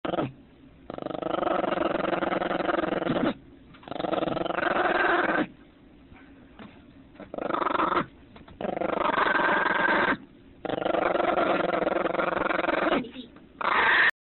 Angry Puppy
tried to take her bone and she got mad